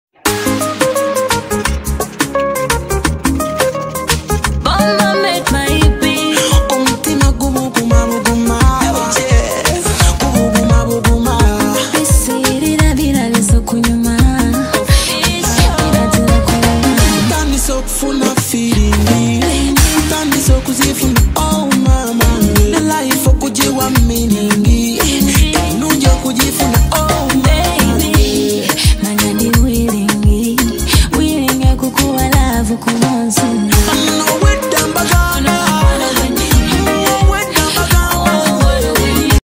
USING A WOODEN SPOON $ A WHISKER TO MIX INGREDIENTS..//Daphiny Maria
Who said cake cant be made without a mixer.🤔. Well here is my mini coconut and orange  layered cake mixed with a wooden spoon and a whisker. Happy birthday july babies.